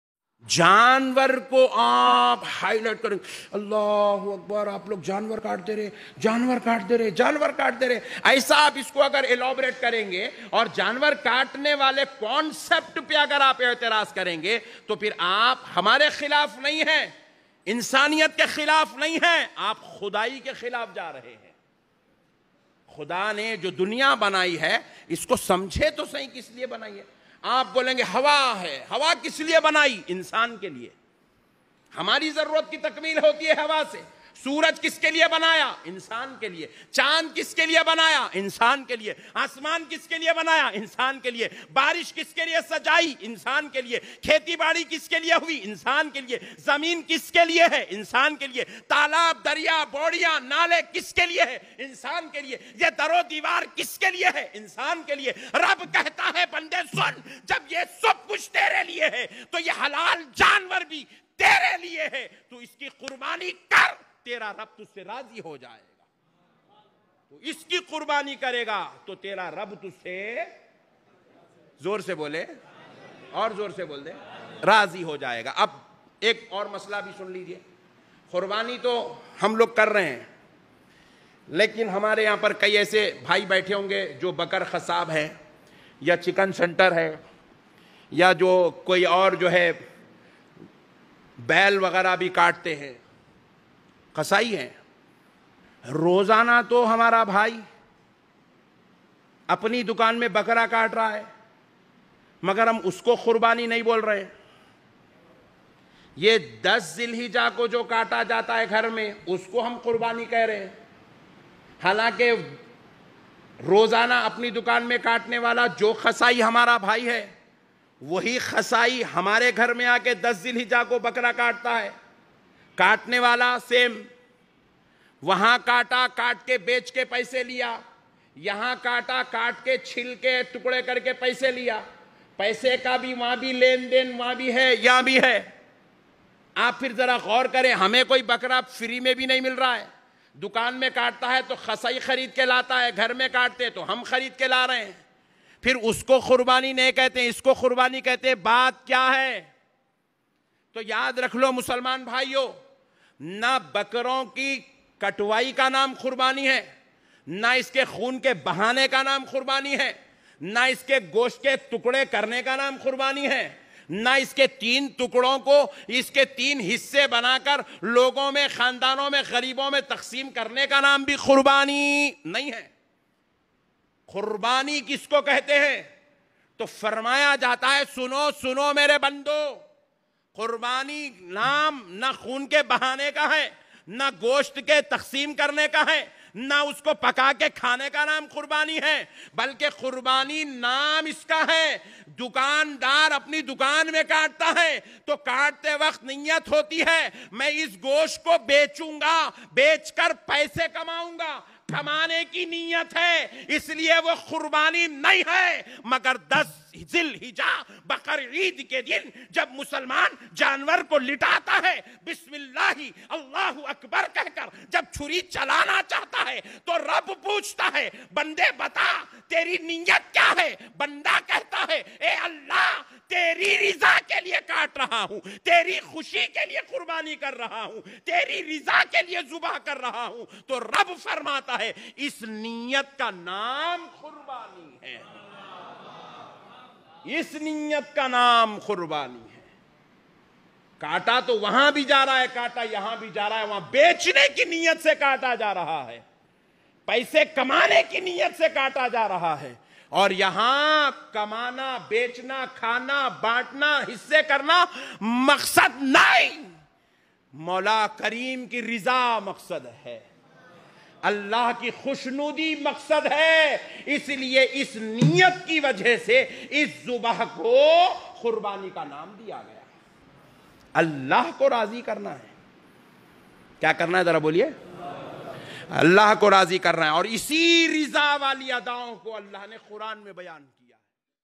Islam me Qurbani Dena kyun Zaroori Hai ? Alama Ahemad Naqshbandi sab
daily new video upload Honge Hamare channel ko support kijiye  Jyada Se Jyada Sher kare..
   . jazak allha kairan